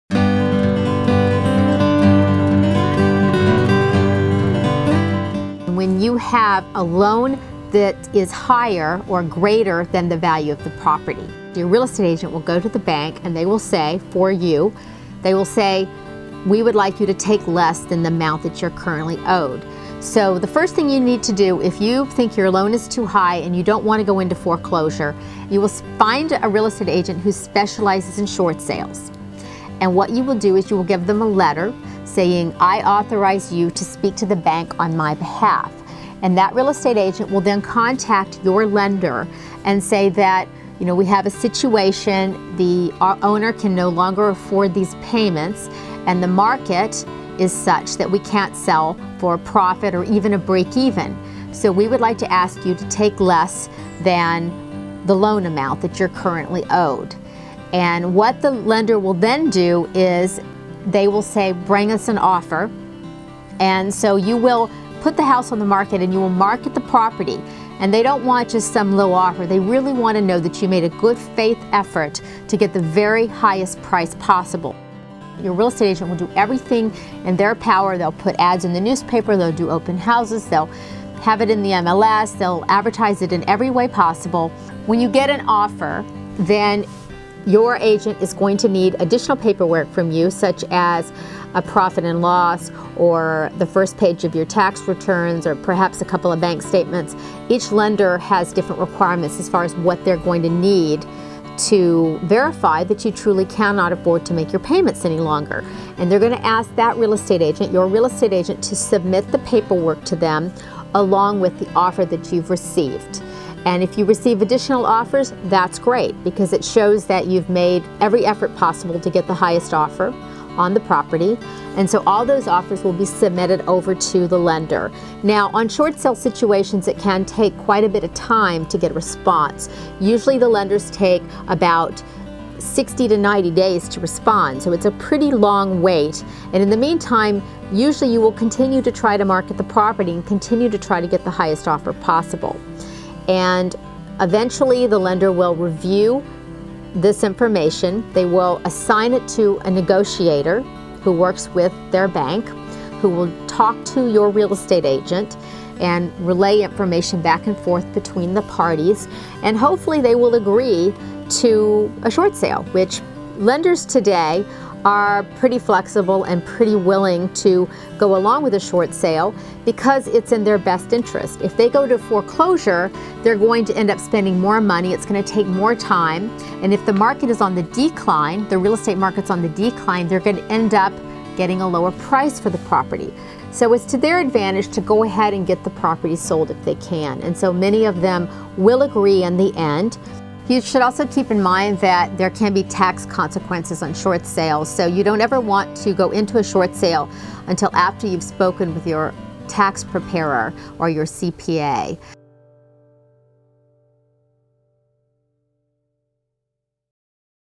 Charlotte Laws explains the Short Sale Process
Charlotte Laws of Berkshire Hathaway HomeServices California Properties (Encino office) explains how short sales work. Her DRE # 01005771. Company DRE # 01317331.